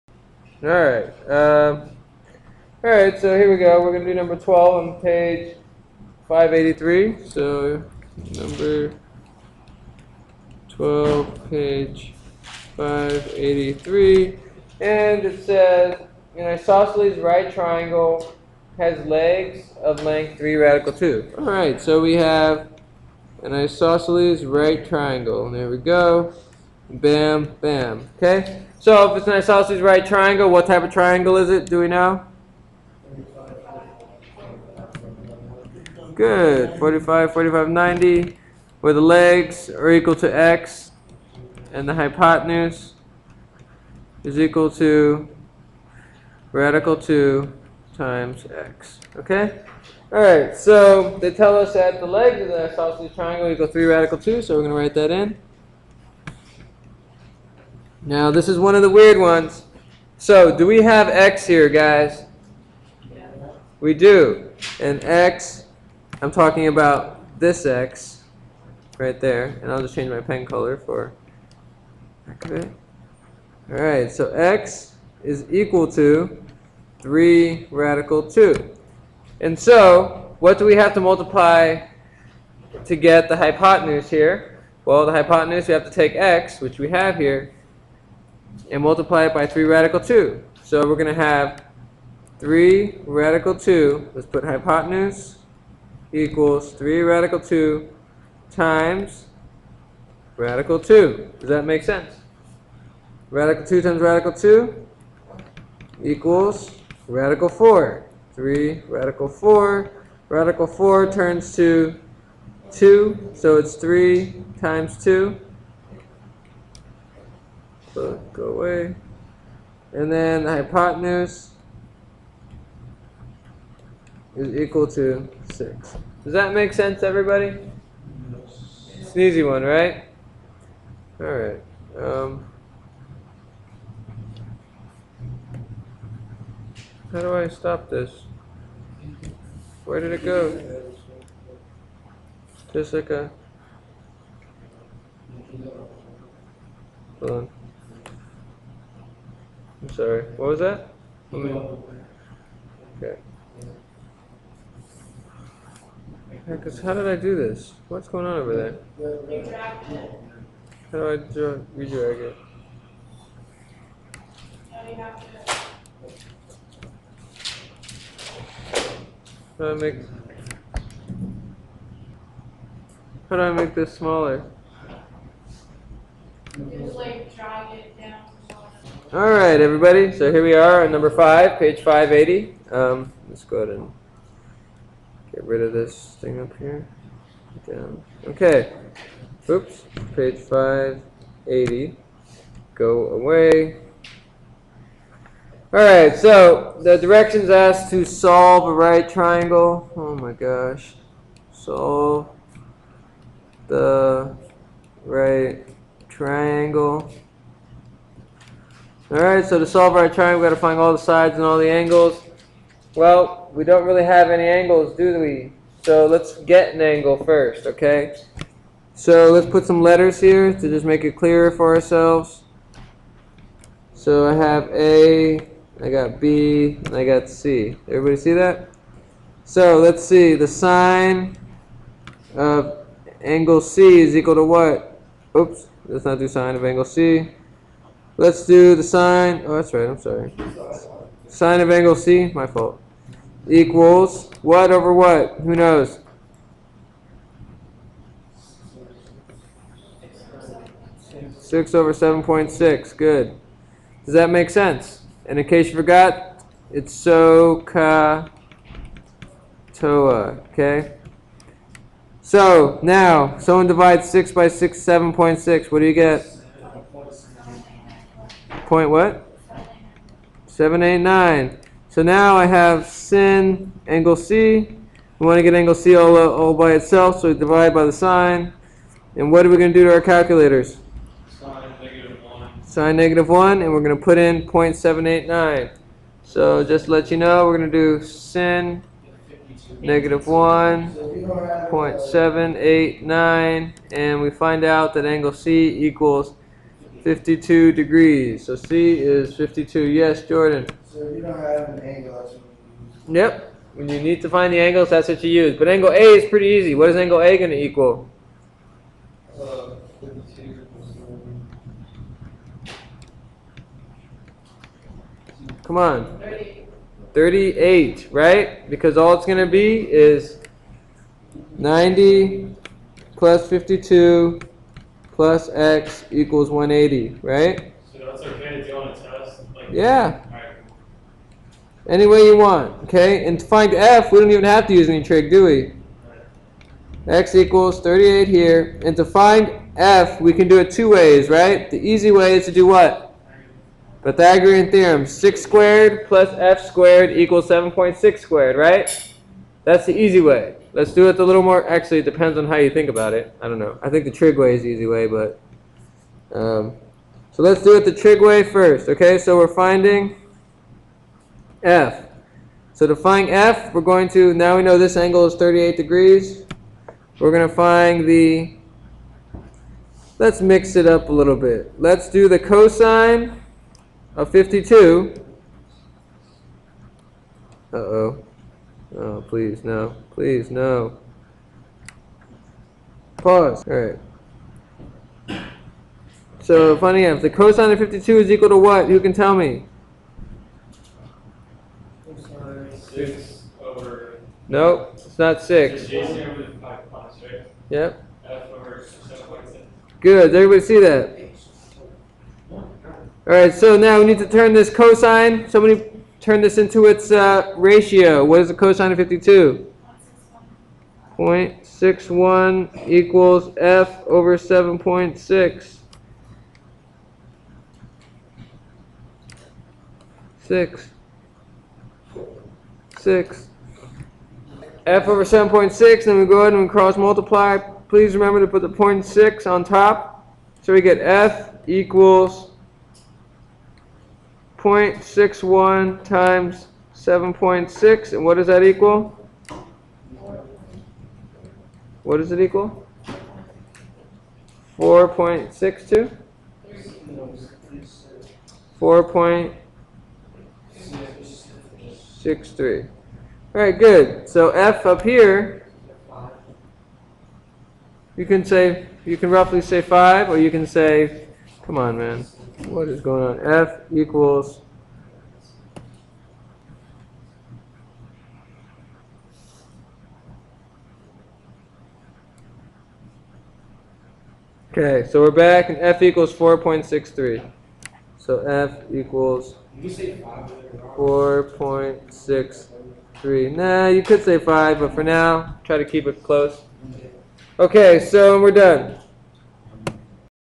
45-45-90 triangles and trig problem
Mr. Yee doing a few more problems with the tablet.